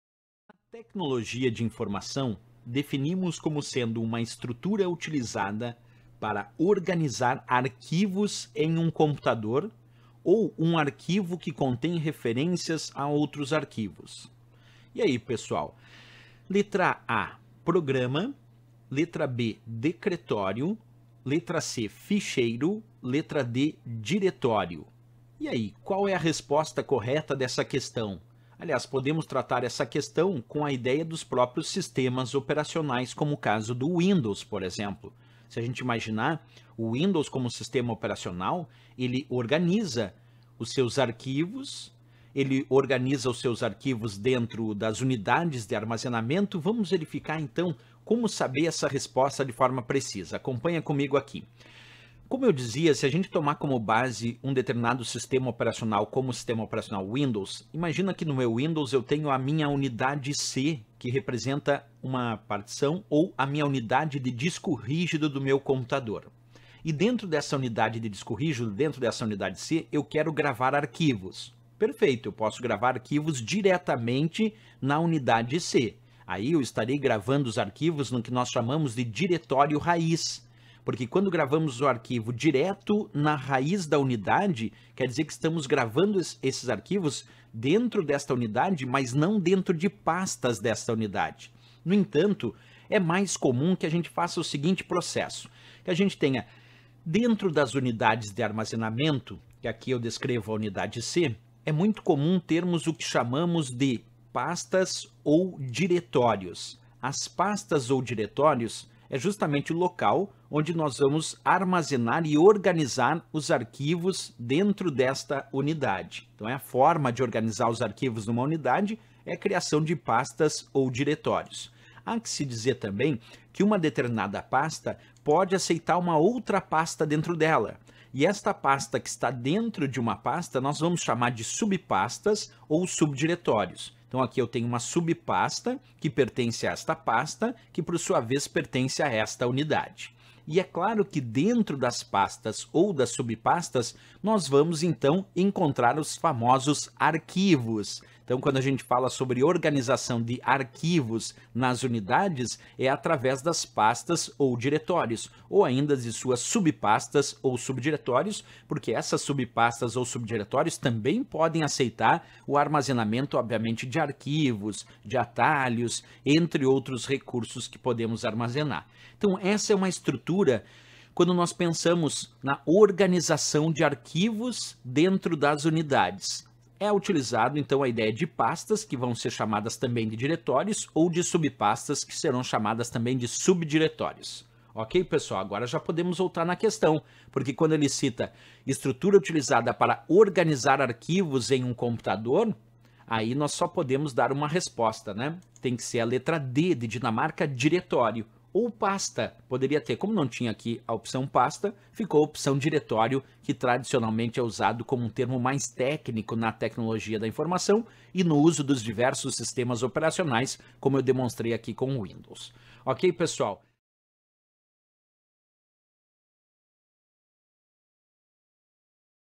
Questões Windows Concursos | Sistema Operacional | Aula 91
Aula 91 sobre o sistema operacional Windows. O professor resolve uma questão sobre a organização de arquivos e pastas.